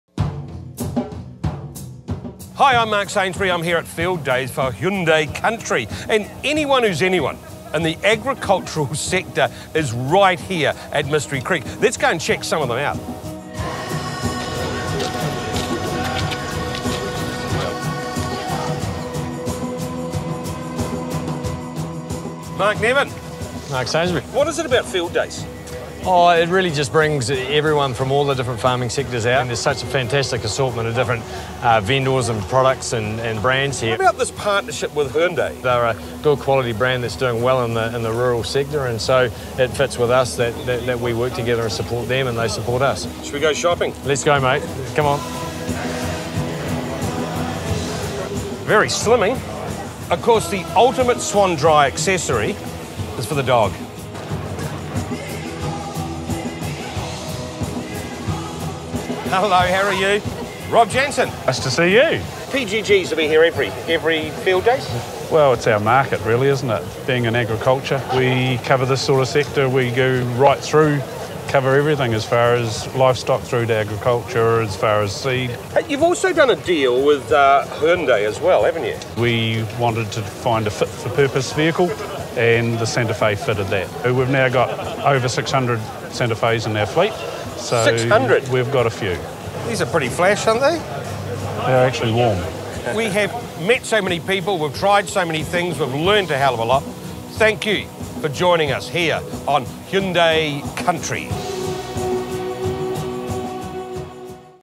National Fieldays - Partnerships with Mark Sainsbury | Hyundai NZ
Mark Sainsbury checks out Fieldays 2015 and how the event bring together a raft of organisations servicing the rural sector.

Follow Mark as he stops to chat with Swanndri NZ and PGG Wrightson and digs a little deeper into their relationship with Hyundai NZ.

Visit the Family Time Project Website for family tips and useful videos